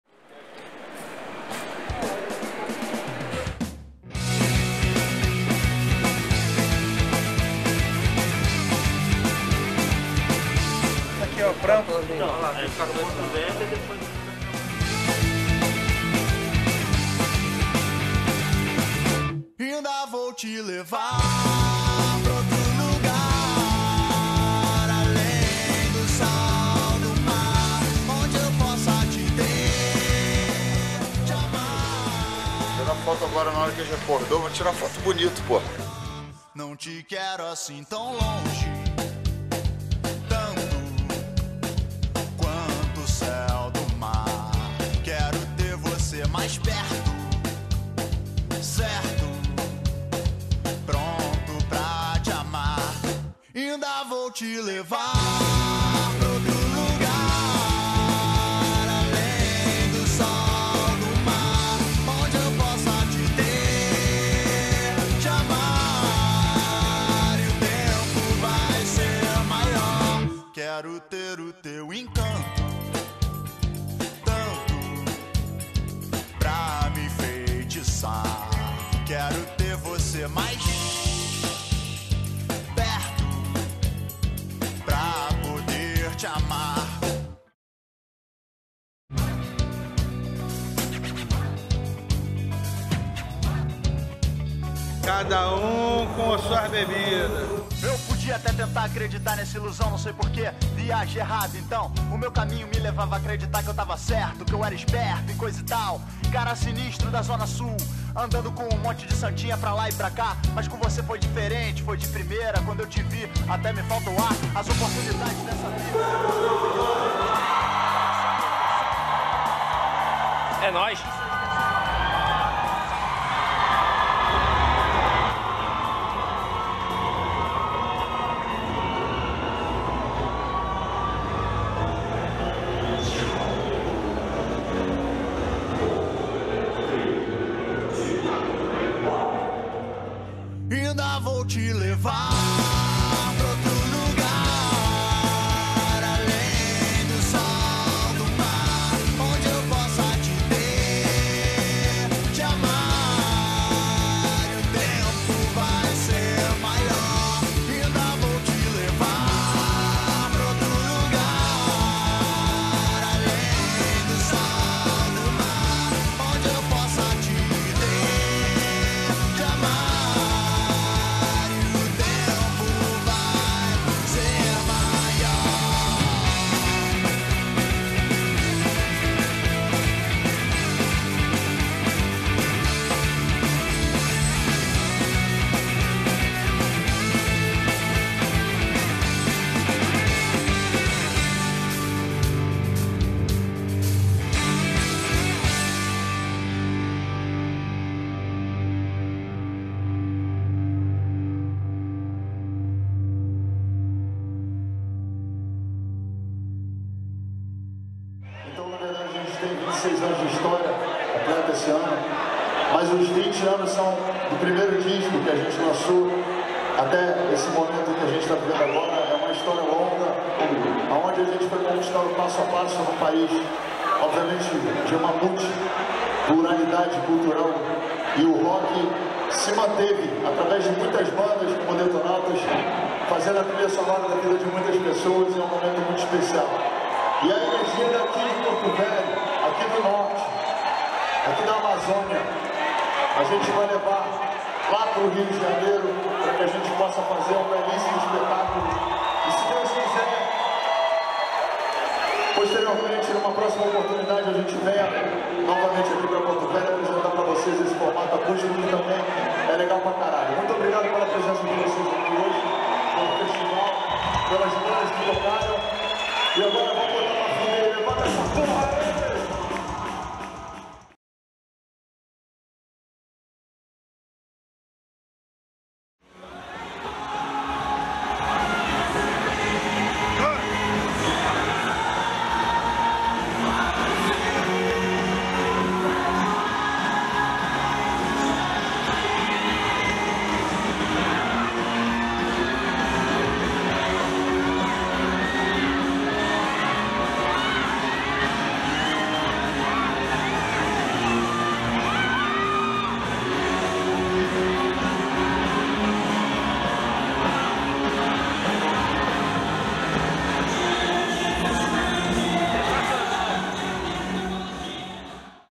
DETONAUTAS NA ESTRADA #53 - Porto Velho/RO (Casarão)
Resumo do show que rolou dia 04/02/23 no Casarão em Porto Velho/RO!

Captação/Edição :
FAAB SANTOS (@santosfaab)

* SIGA O DETONAUTAS *